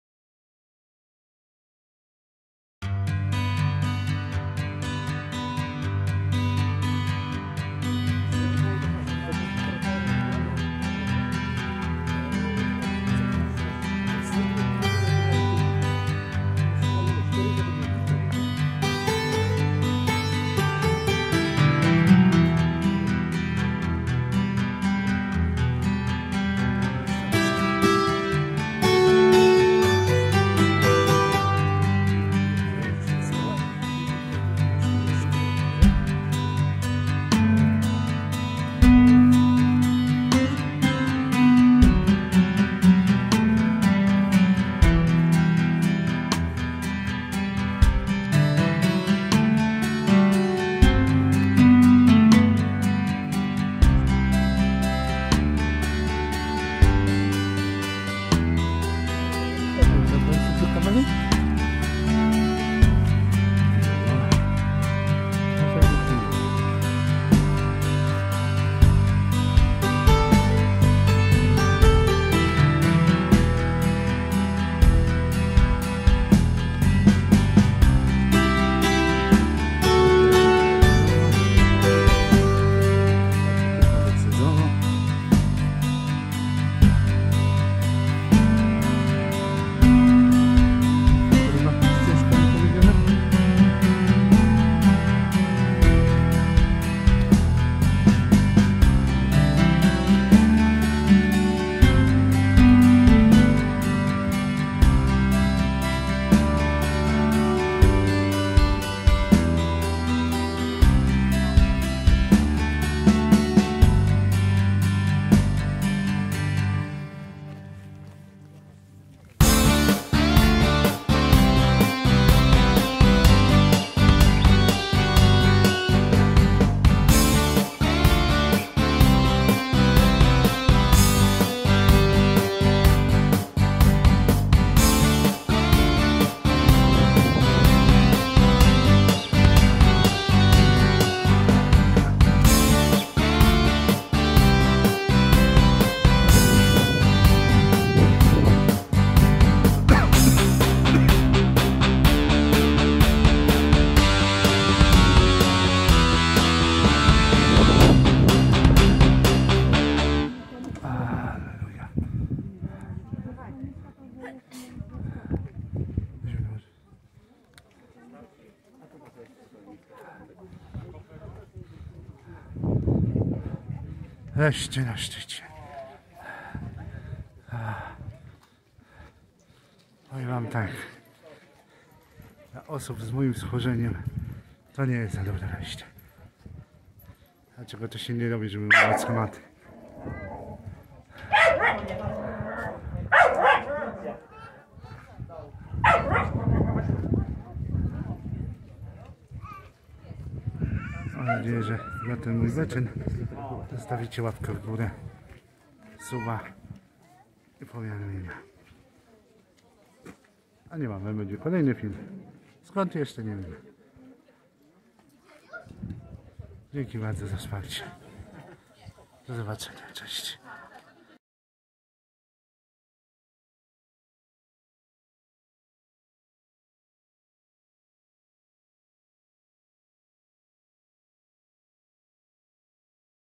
Karpacz - Dolina Śląska - Śnieżka - zdobycie szczytu
Śnieżka (cz. Sněžka, niem. Schwarzkopf) – najwyższy szczyt Karkonoszy oraz Sudetów, jak również Czech, województwa dolnośląskiego, a także całego Śląska. Najwybitniejszy szczyt Polski i Czech (MDW 1203 m). Jego oficjalna wysokość bezwzględna to 1602 m n.p.m. według Karkonoskiego Parku Narodowego, a także najnowszych pomiarów wartość ta wynosi jednak 1603,296 m n.p.m.